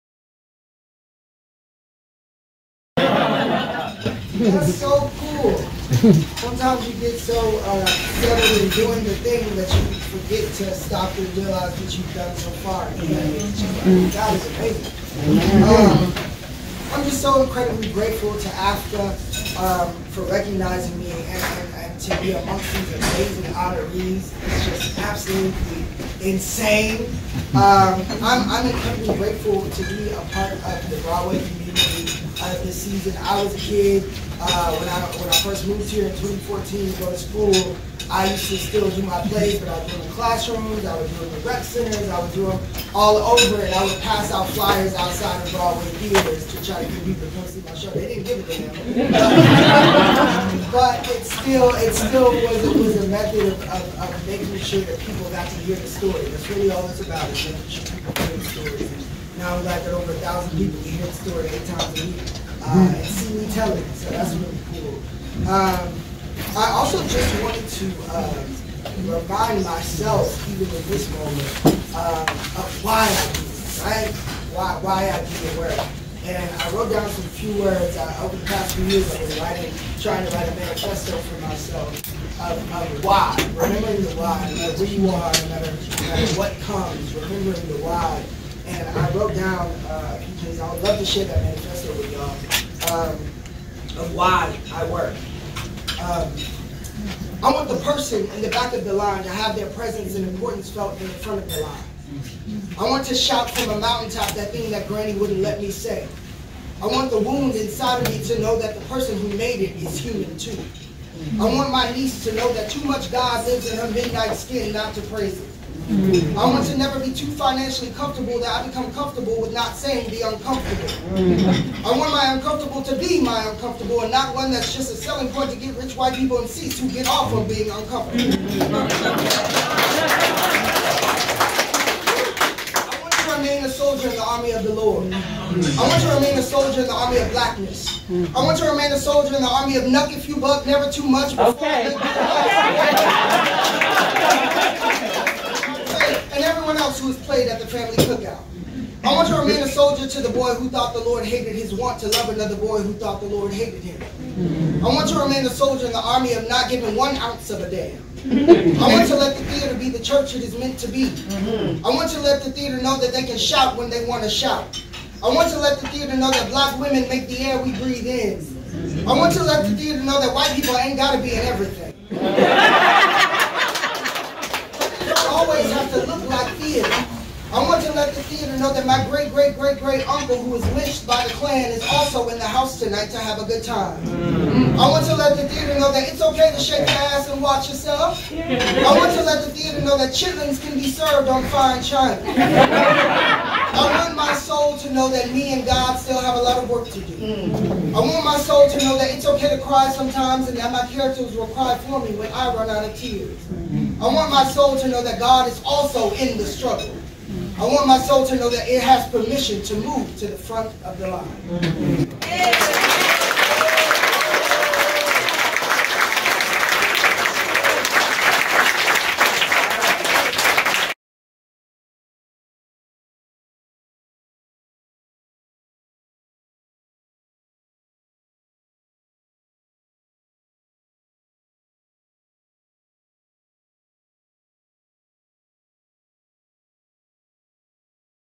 Jordan E Cooper Breakout Creative AAFCA Award Acceptance Speech
Jordan E Cooper is a broadway Playwright and the African American Film Critics Association celebrates his talent at the AAFCA Salutes Broadway Awards on Monday, October 17th at the Lamb’s Club in New York’s theater district.

AAFCA Salutes Broadway with its first Awards ceremony to highlight the great talent that brings life to the stories told on stage every night. Jordan E Cooper Accepted the Emerging Creative Award and read from his manifesto.